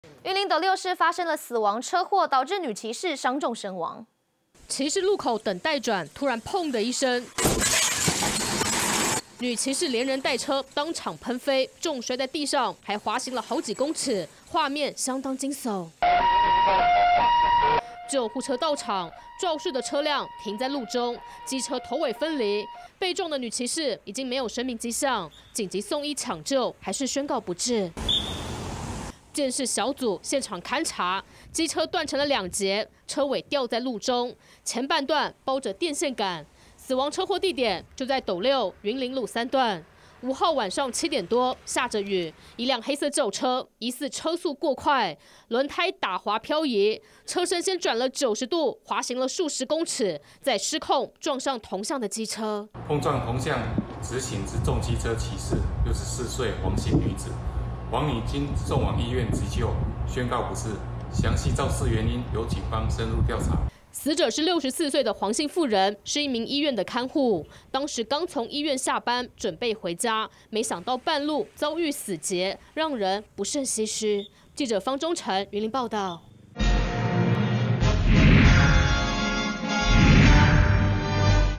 轎車疑打滑失控暴衝 女騎士遭撞飛身亡
雲林斗六市發生死亡車禍，一輛黑色轎車疑似因為天雨路滑，加上未減速輪胎打滑飄移，高速撞上同向的機車騎士。女騎士連人帶車噴飛，當場失去呼吸心跳，送醫後不治身亡。據了解這名騎士是醫院看護，下班途中卻遭遇死劫。

下載【年代電視】APP：
iOS平台：